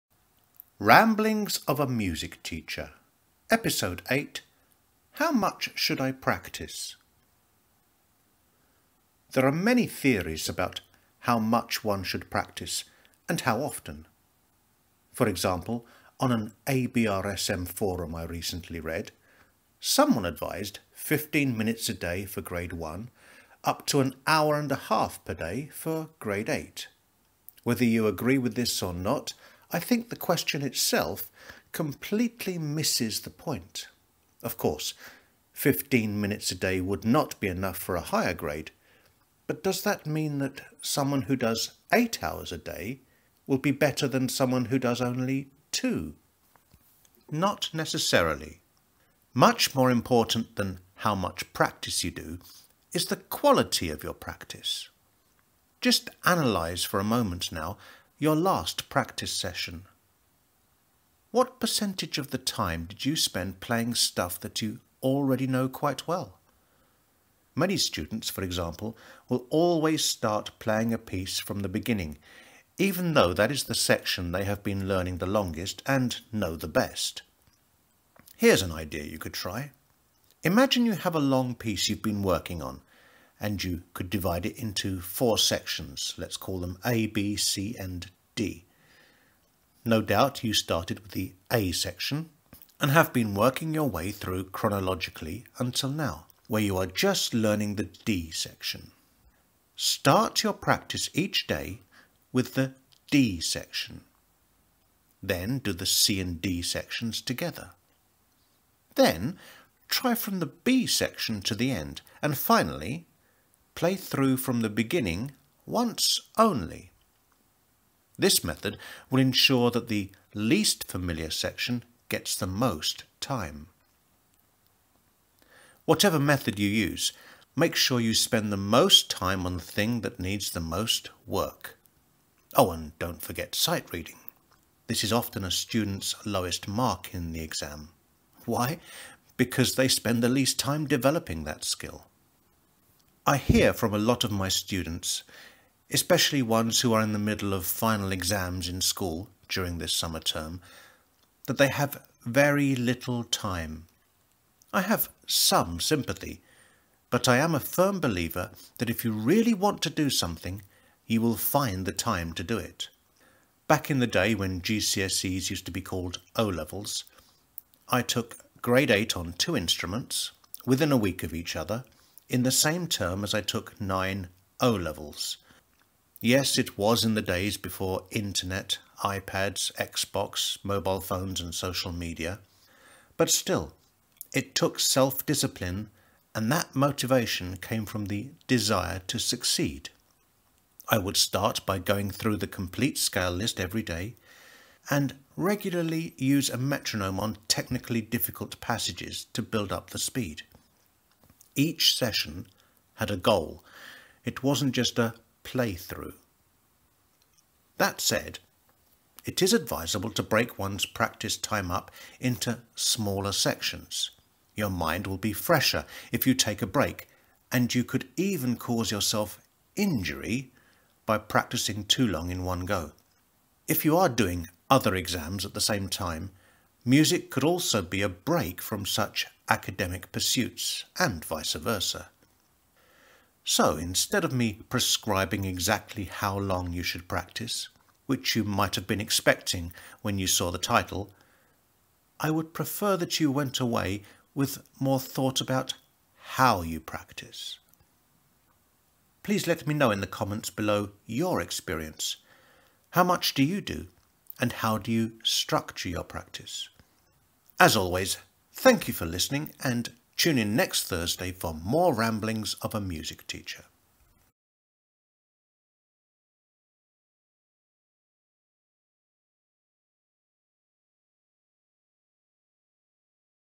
How much should I practise? - MusicOnline UK Podcast - Episode 8
Want a DISTINCTION in YOUR next ABRSM Exam? There are many theories about how much one should practice and how often. For example, on an ABRSM forum I recently read, someone advised 15 minutes a day for Grade 1, up to an hour and a half per day for Grade 8.

However, I think this question itself completely misses the point. Of course, 15 minutes a day would not be enough for a higher grade, but does that mean that someone who does 8 hours a day will be better than someone who does only two?

Not necessarily.